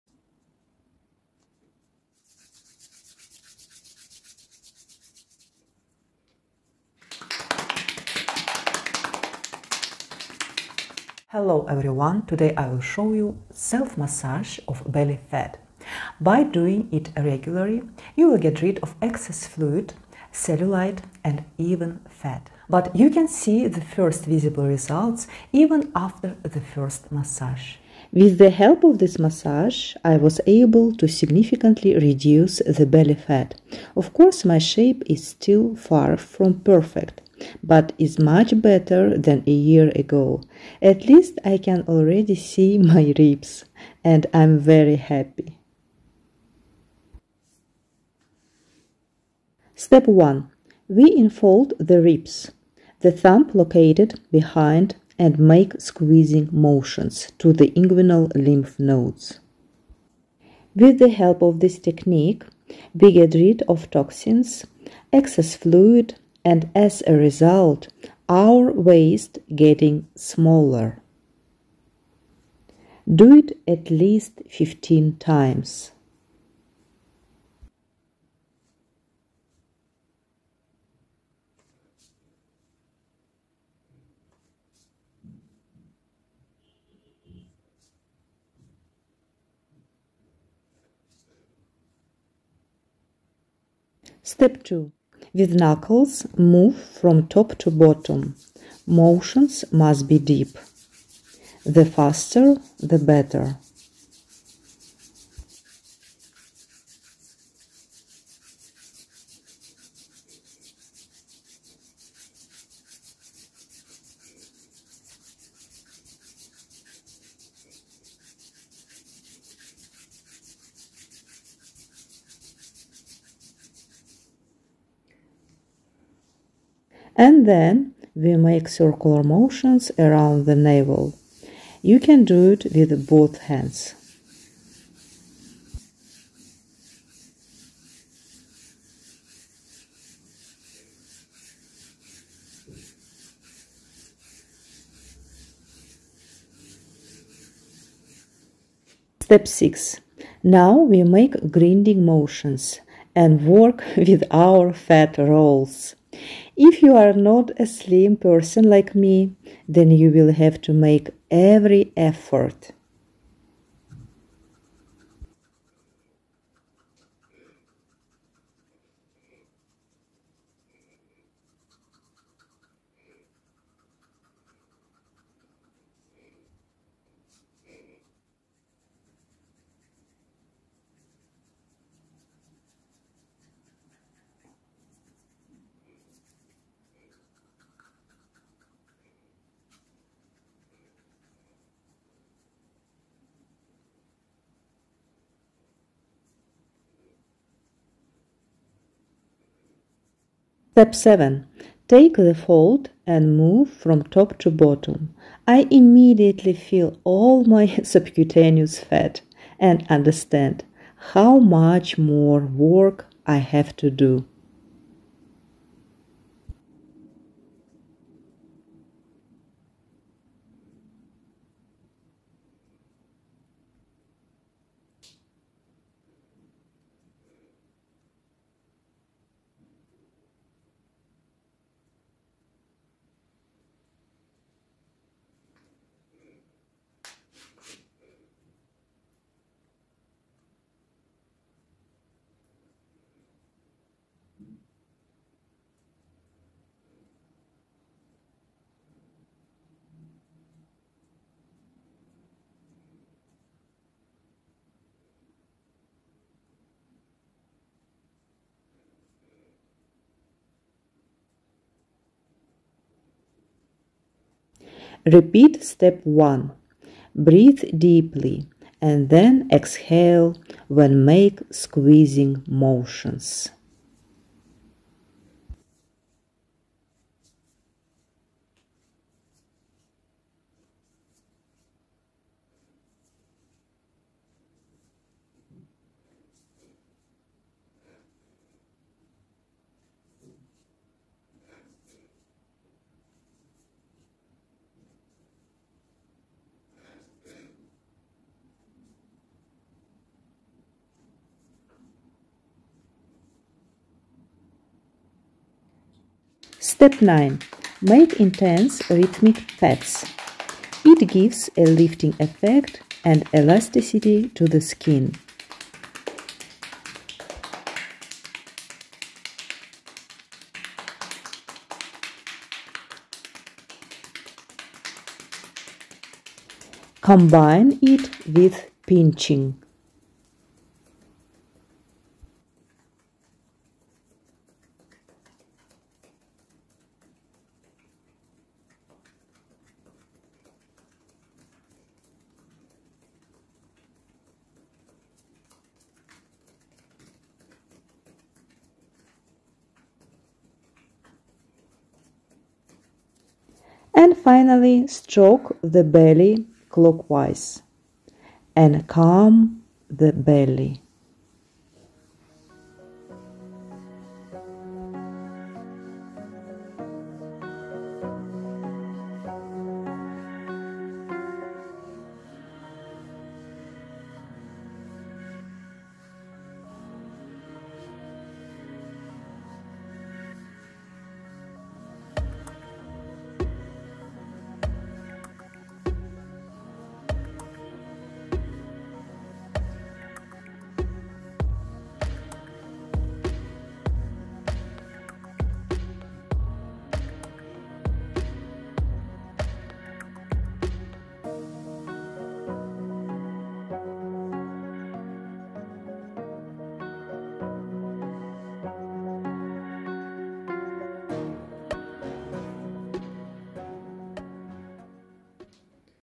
Massage your BELLY FAT for 6 minutes to make it FLAT
With the help of this belly massage you will get rid of belly fat and make your stomach flat and slim and get rid of cellulite. Do this massage and make your belly flat and slim